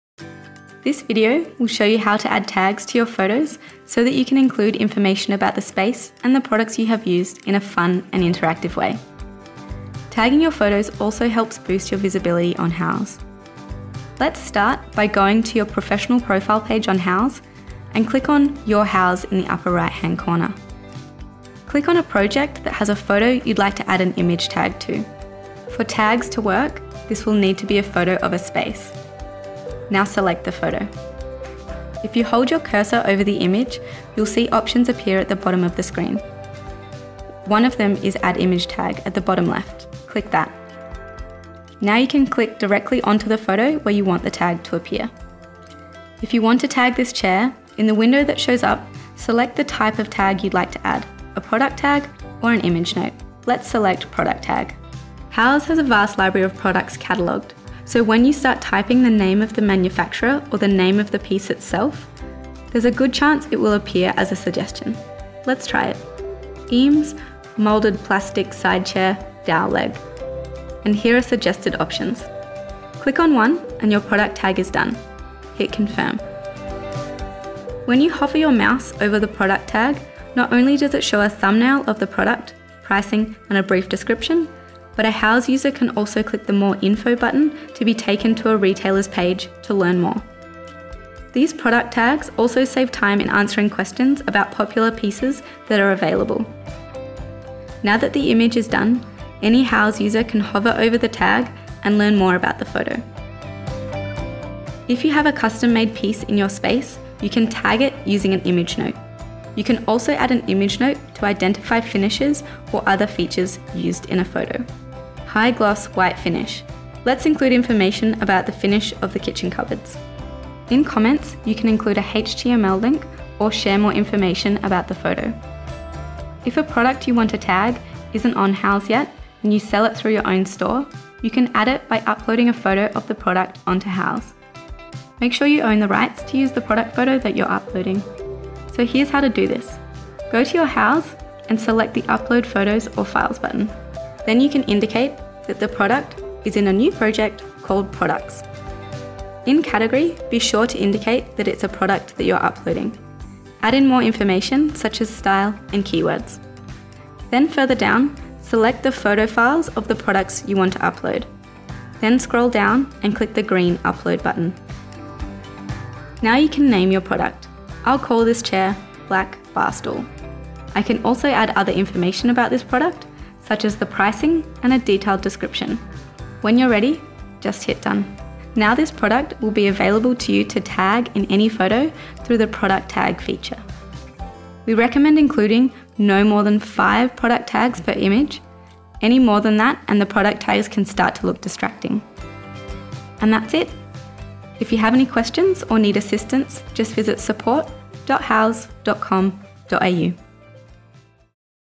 Houzz For Pros: How To Tag Photos
Learn how to answer questions about your photos proactively by tagging photos in your portfolio with product information.